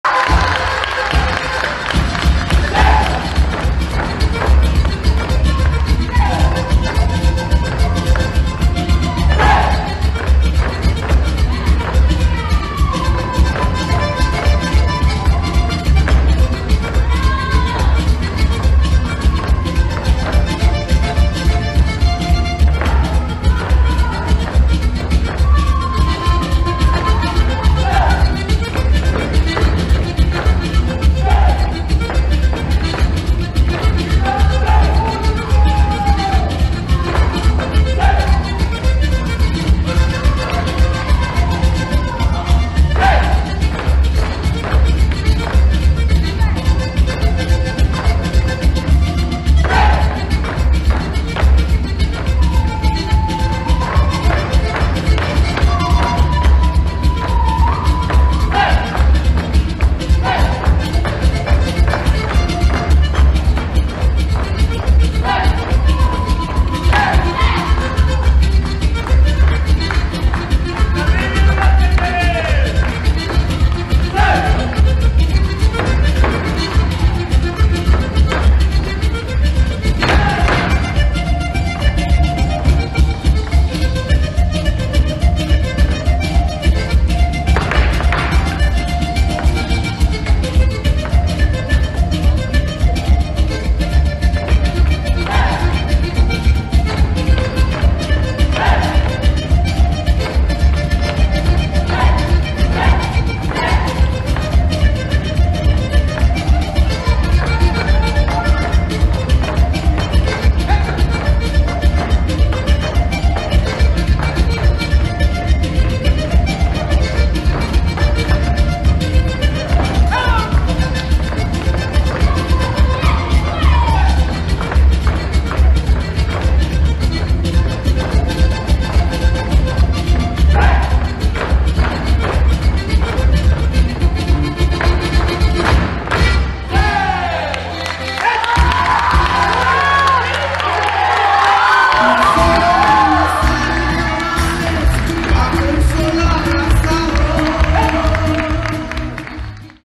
Calabaceados - Baja California 4
Compañía de Danza Folclórica independiente Yolotl Macehuani presentes en el Encuentro Nacional de Danza Folklórica “Tierra de mis amores” Guanajuato.
En el Centro Cultural Mariel.